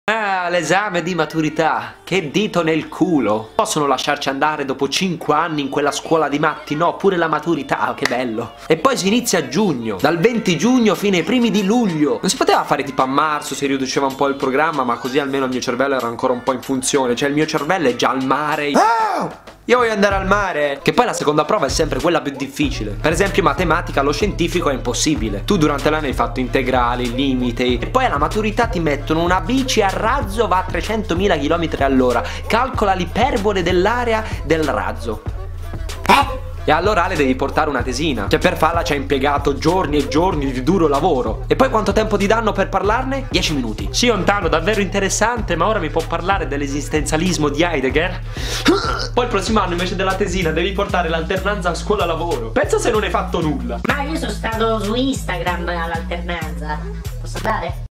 IO ODIO LA MATURITA - Edoardo Ontano
Ci siamo! Domani è il grande giorno e stiamo piano piano morendo dentro! UNIAMOCI E VINCIAMO!